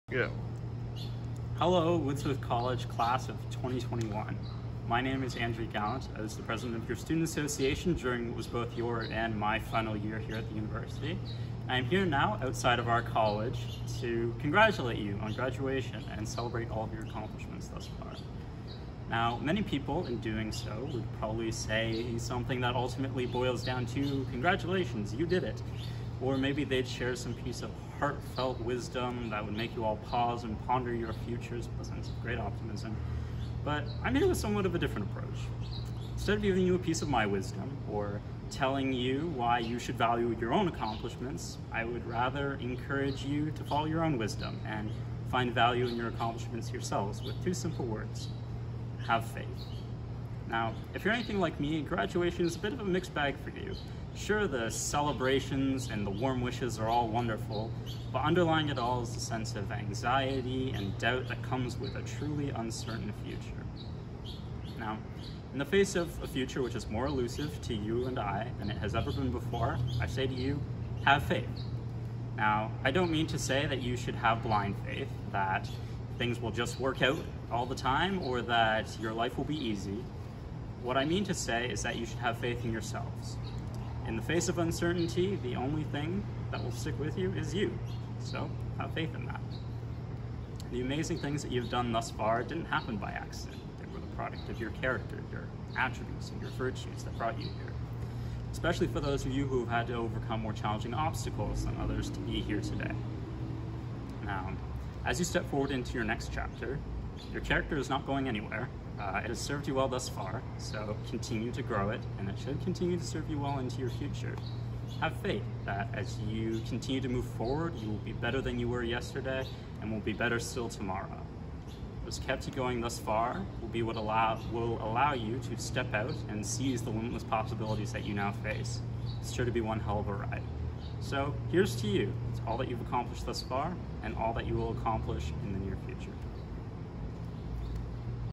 Andrew Gallant Convocation Message
A message from the former president of the Woodsworth College Students' Association